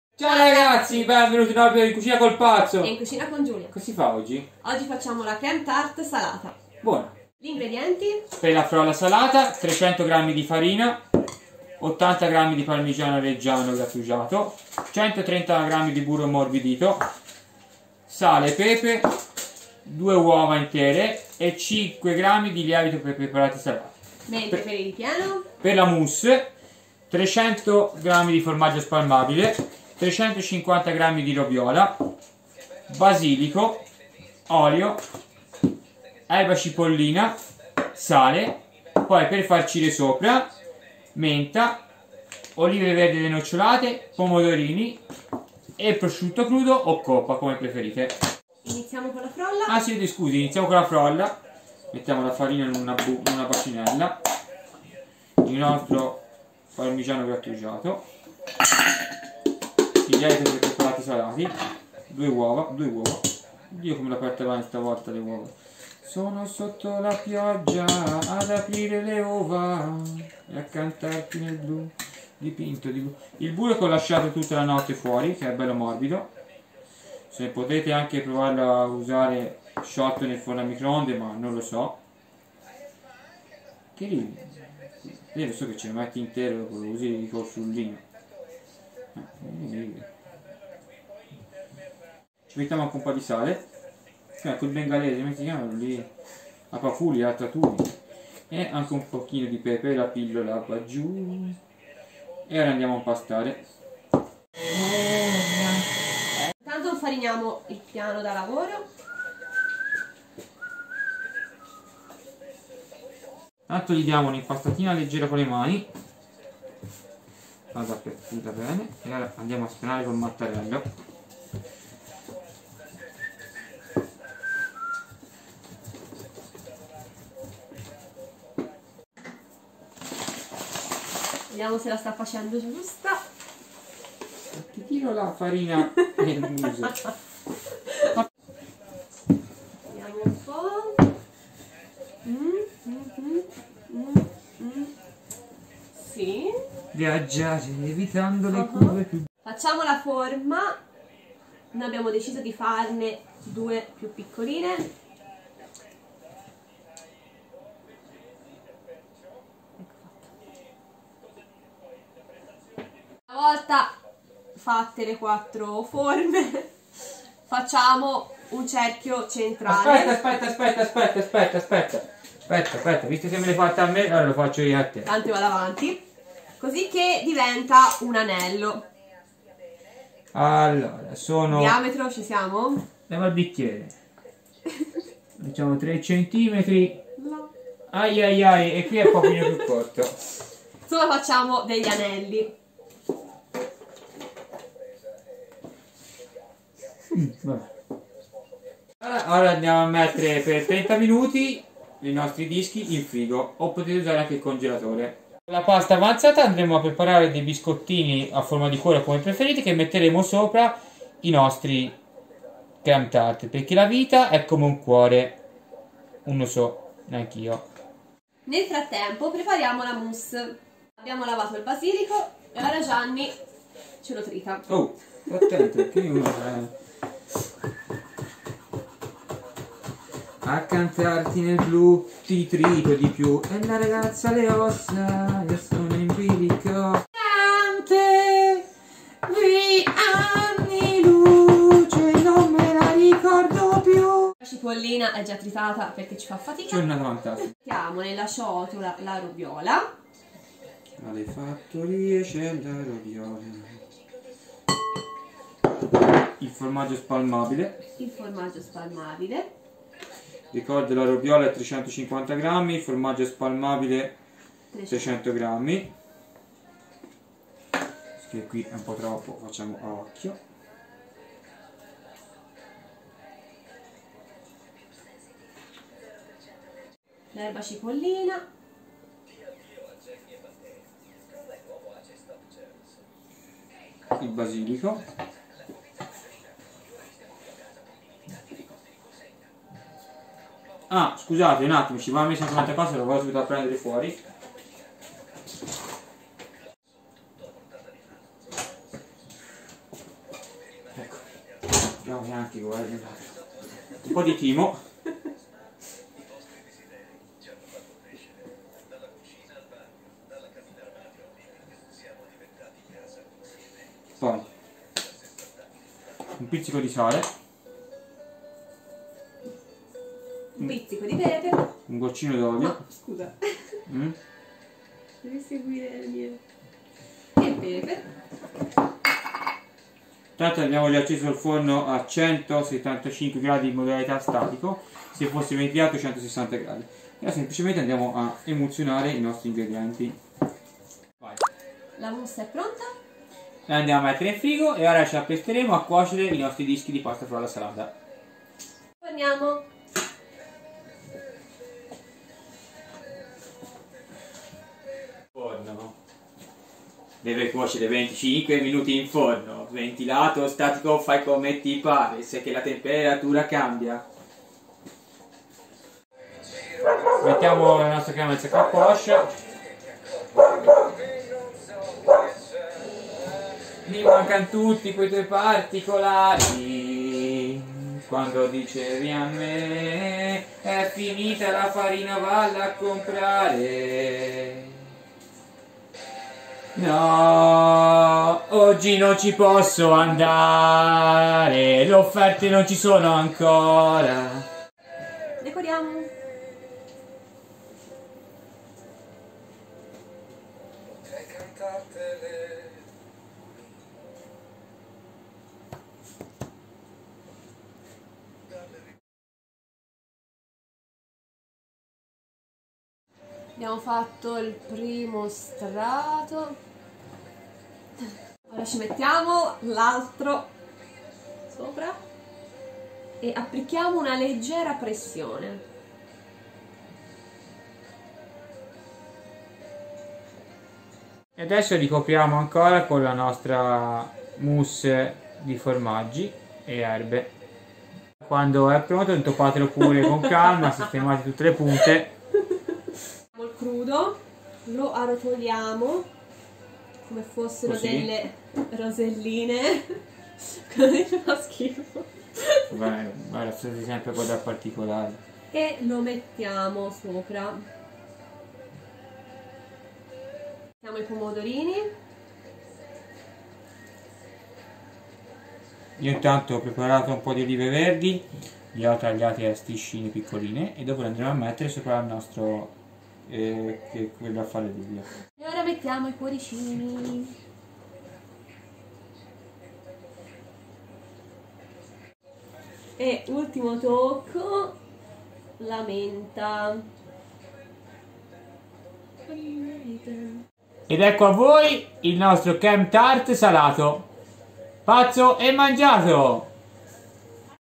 Creme tart salata
PER LA FROLLA SALATA
Parmigiano Reggiano grattugiato 80 g
uova medie 2
lievito in polvere per torte salate 5 g
sale fino
pepe macinato a piacere
PER LA MOUSSE
robiola 350 g
formaggio spalmabile 350 g
erbe cipollina
basilico, timo
PER FARCIRE
Prosciutto crudo
pomodorini  rossi
olive verdi
menta qualche fogliolina